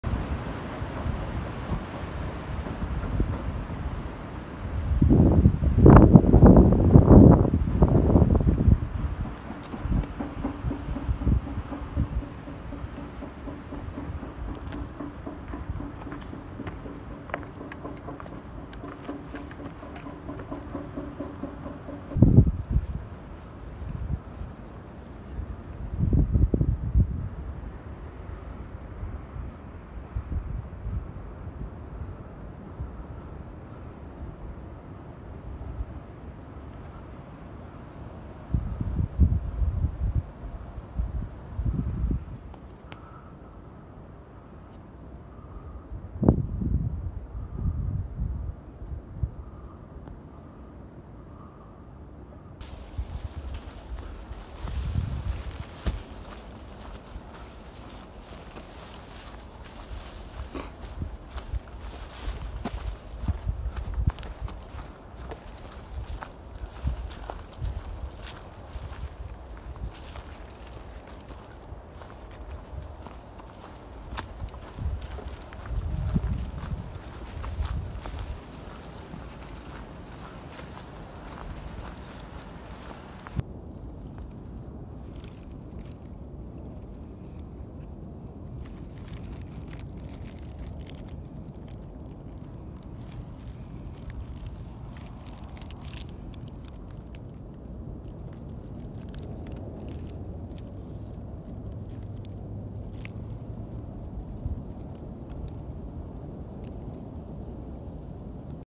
高川山〜むすび山
2020/03/24 カタクリ見頃。
高川山からむすび山へ
初狩駅から大月駅まで縦走にて